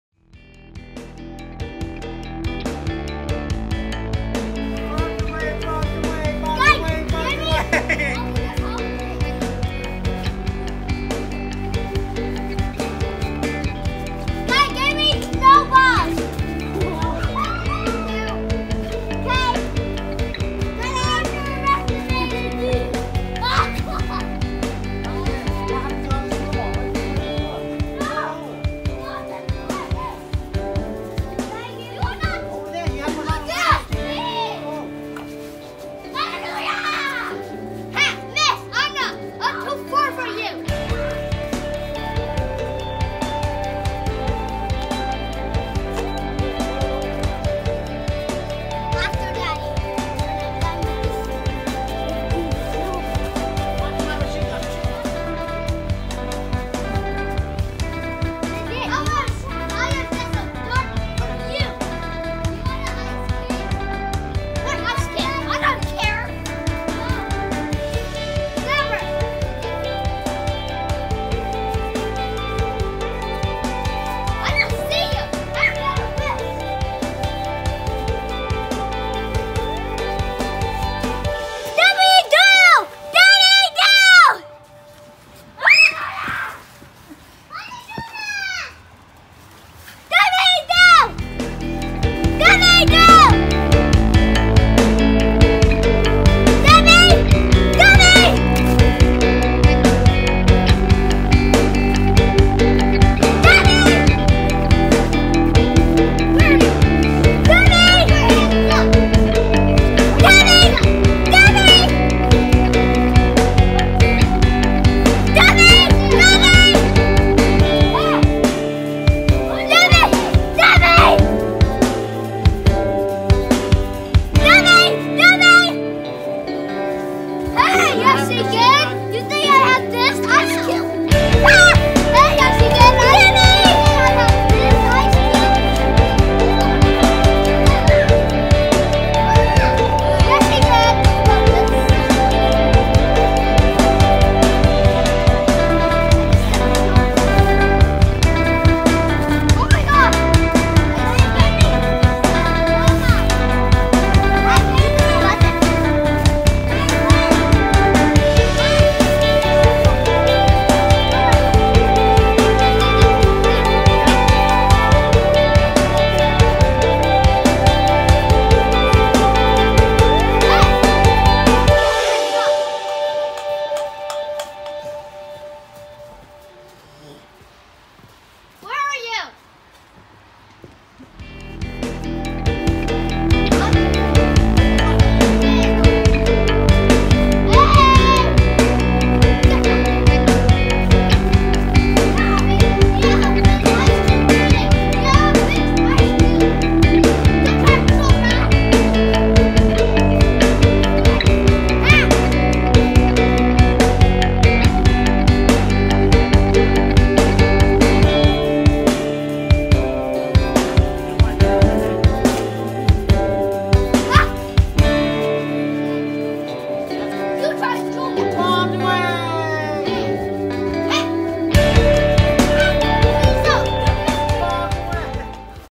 Neighbor Snowball Fight
The kids are having fun throwing the snowballs with Neighbor’s Kids. They actually are friends, go to the same school and in the same class too. 👍🏼☺️